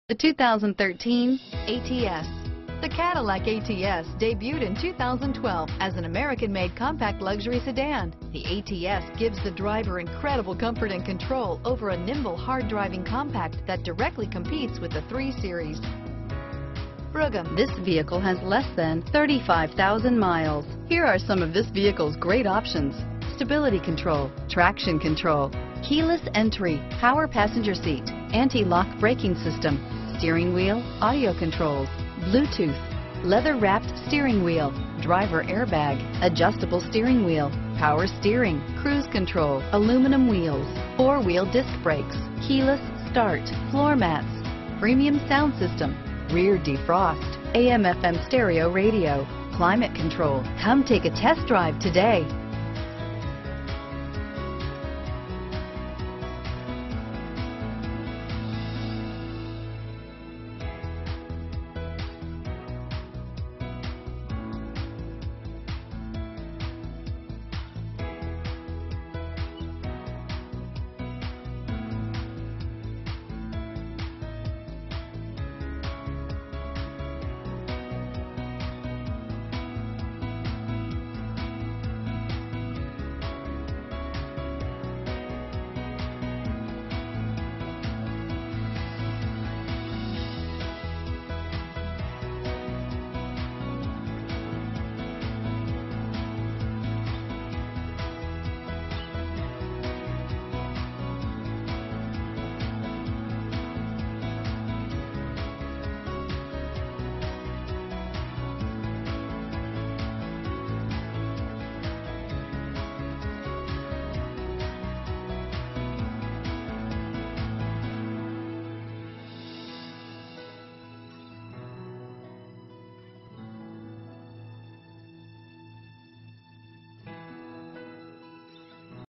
2013 Cadillac ATS Orange County, Irvine, Laguna Niguel, Newport Beach, Mission Viejo, CA 8839
2013 Cadillac ATS
2013 Cadillac ATS 4DR SDN - Stock#: 8839 - VIN#: 1G6AA5RA1D0129741

CADILLACS FINEST CERTIFIED PRE OWNED DIAMOND!! Take this one out of early retirement. Lo-Lo-Miles! Your quest for a gently used car is over. This great 2013 Cadillac ATS has only had one previous owner, with a great track record and a long life ahead of it. This fantastic Cadillac is one of the most sought after used vehicles on the market because it NEVER lets owners down.